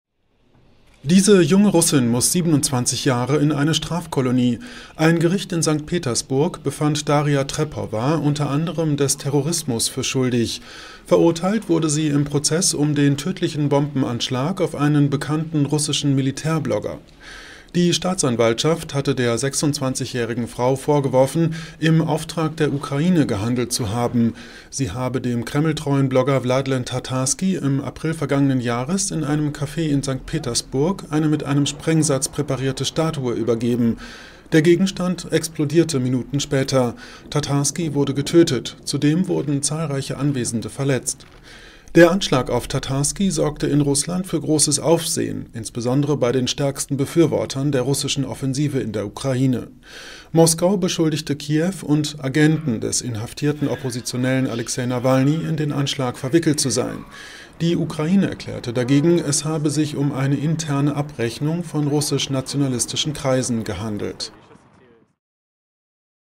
Anschlag auf russischen Militärblogger: Frau zu 27 Jahren Haft verurteilt | AFP
Im Prozess um den tödlichen Bombenanschlag auf einen bekannten russischen Militärblogger hat ein Gericht in St. Petersburg eine Frau zu 27 Jahren Haft in einer Strafkolonie verurteilt. Darja Trepowa sei unter anderem des Terrorismus für schuldig befunden worden, erklärte das Gericht.